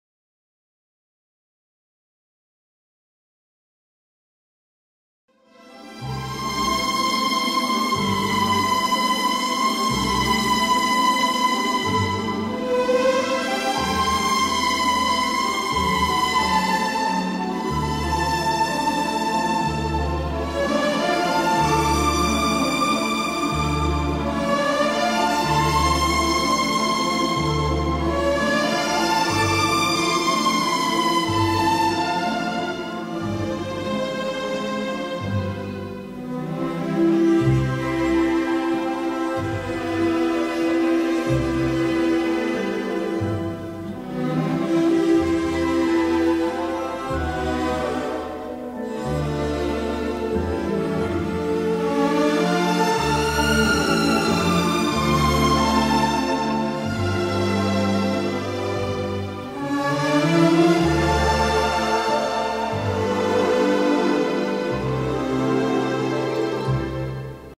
Salamanca - Santa Marta de Tormes - AHR
Santa Marta de Tormes es un pueblo situado en la provincia de Salamanca en Castilla y León, a orillas del río Tormes.